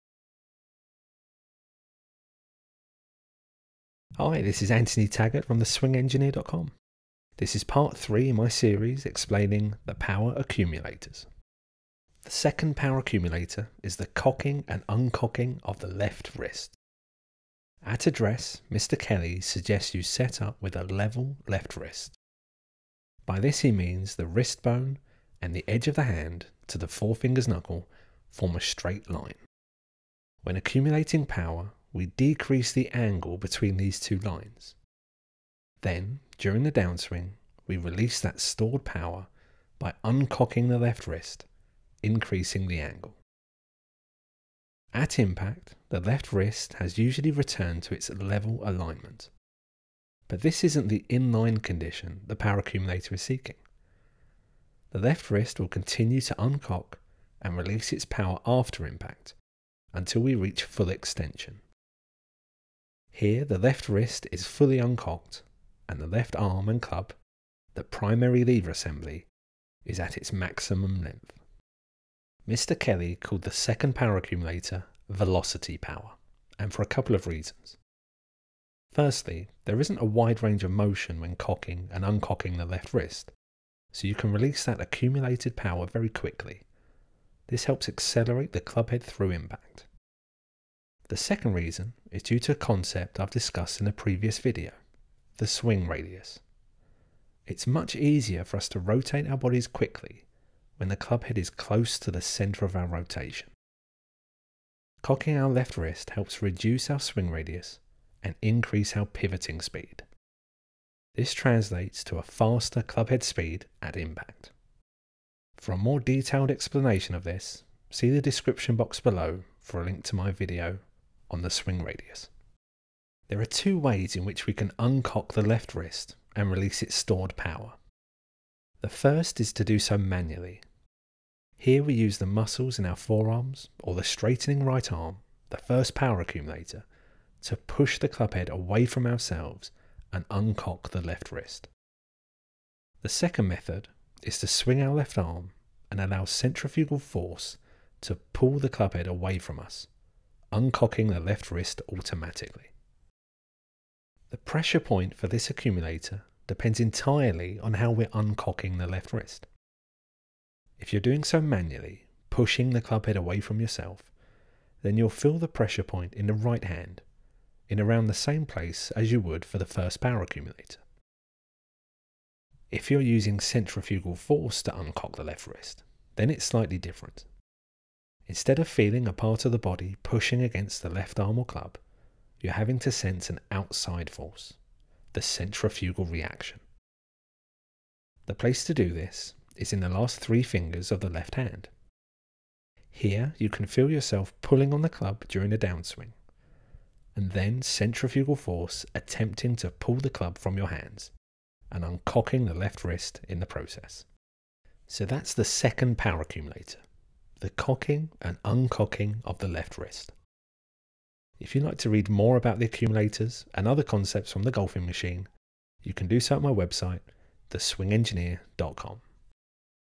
Power Accumulators (Part 3)
In part three of the Power Accumulator series, The Swing Engineer explains how the second power accumulator works.

Visit TheSwingEngineer.com for more details or to purchase your own copy of The Golfing Machine.